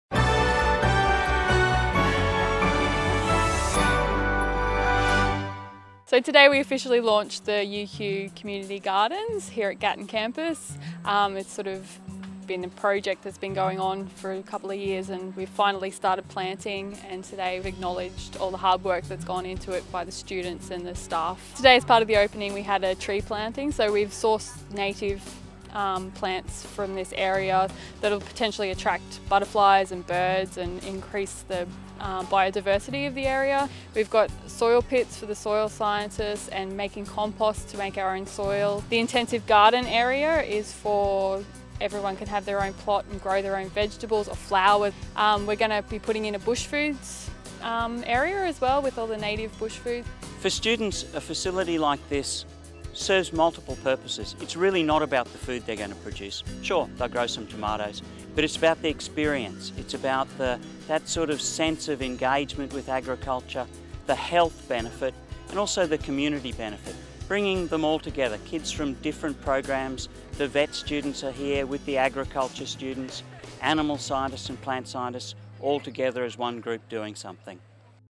UQ Gatton Community Garden
The hard work and creativity of a keen group of University of Queensland students have seen an idea bloom into the successful creation of the UQ Gatton Community Garden.

The UQ Plant Science Society created the garden, which includes a soil pit, outdoor seating and a covered deck that can be used as a versatile classroom.